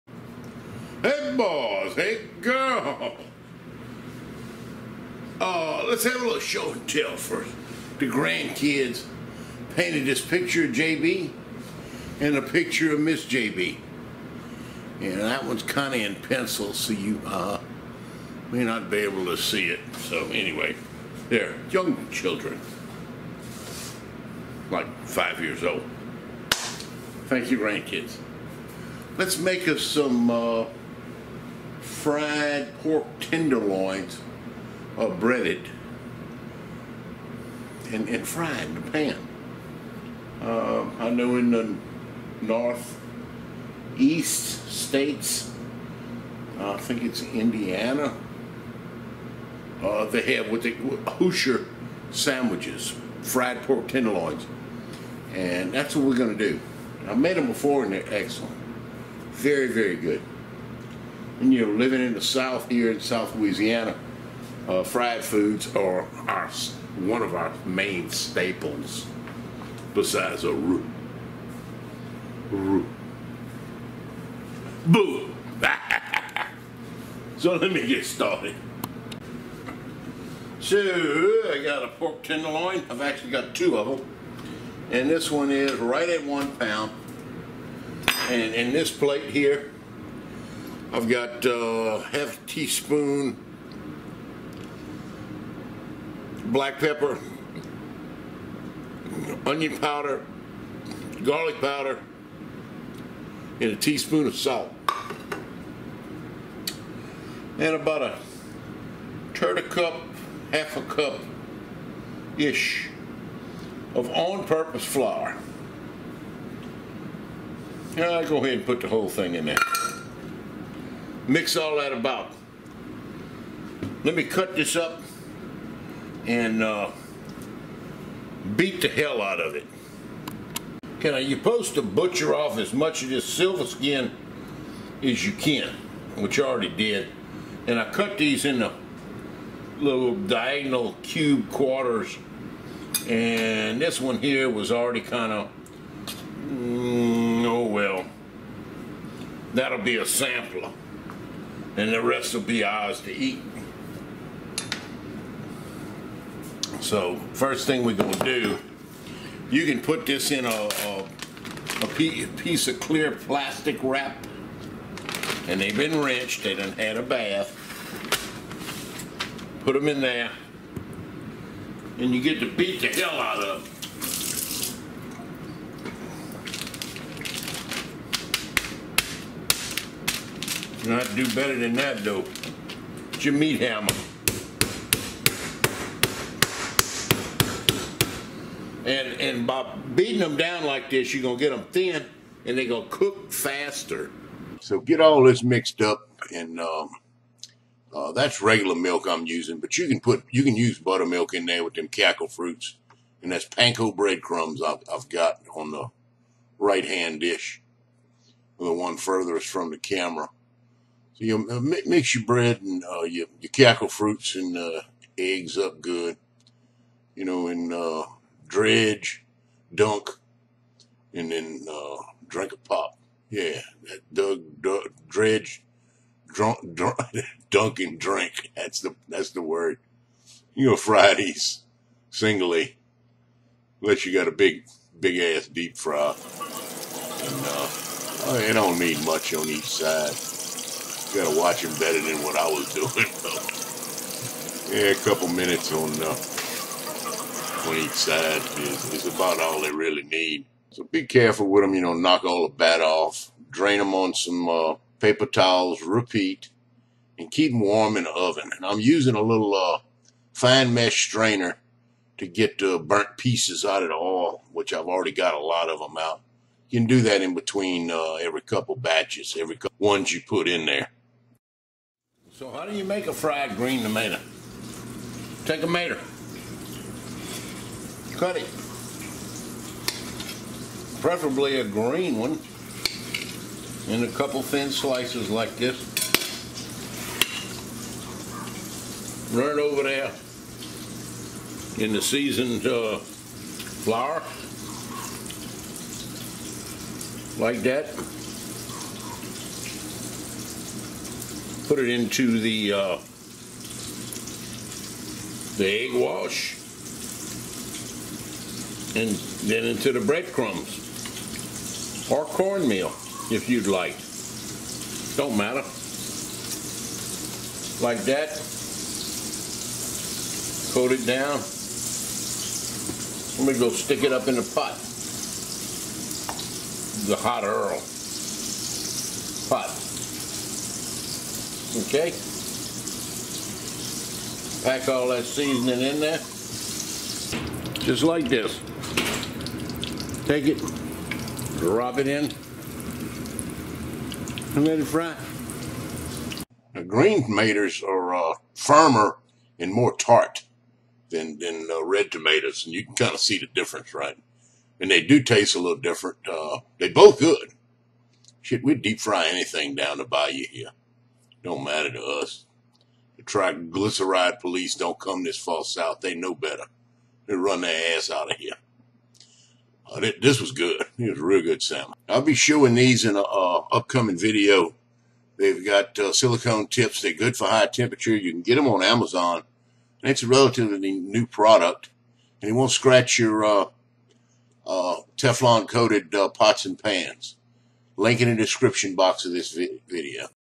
Pork Tenderloin Sandwich - How to make a Hoosier Fried pork Sandwich
With Fried Green & Red Tomatoes Recipe... Link to the Tongs